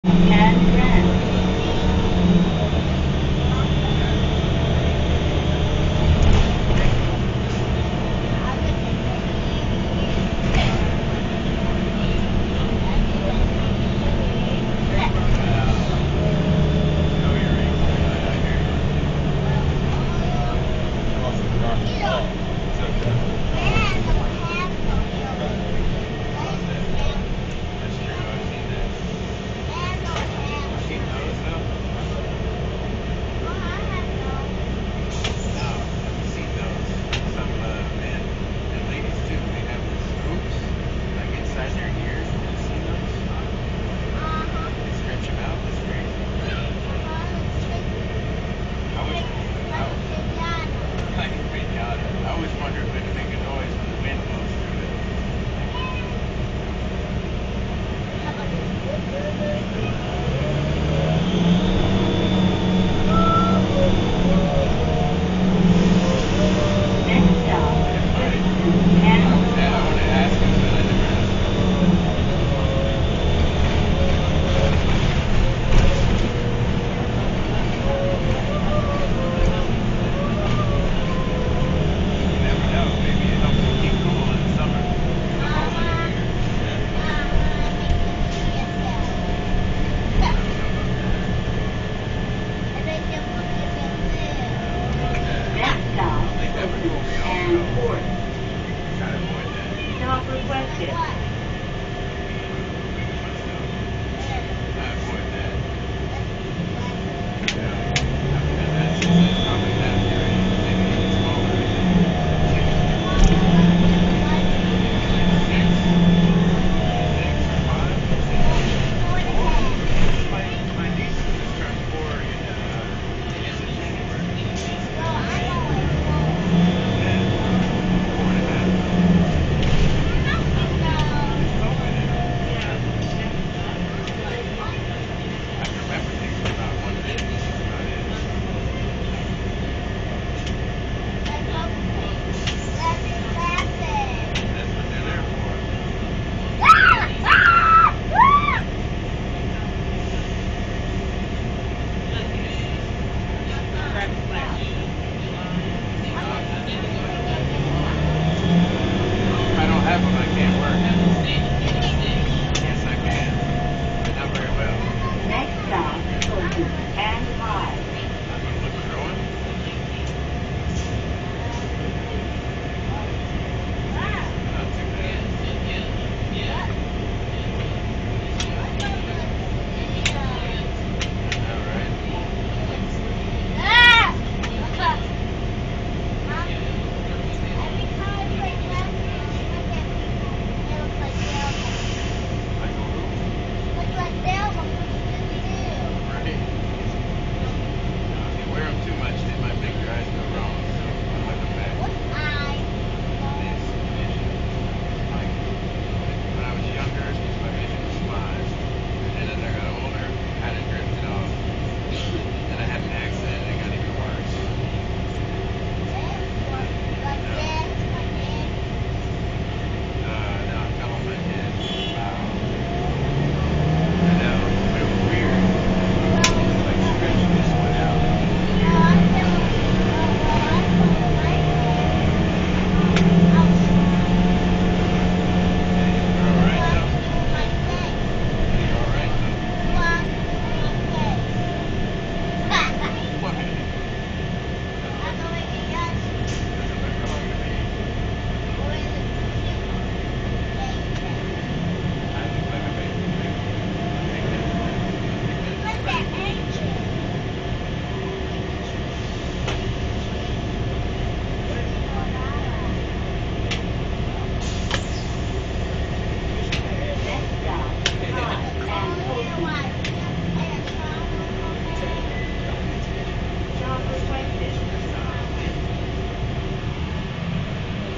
Take a ride on COTA's 2010 Gillig Advantage (with a possible Turbo)
The audio from my first bus sound recording video came from this video.

I have another video of a 2010 Gillig Bus with a turbo that i will release at a later date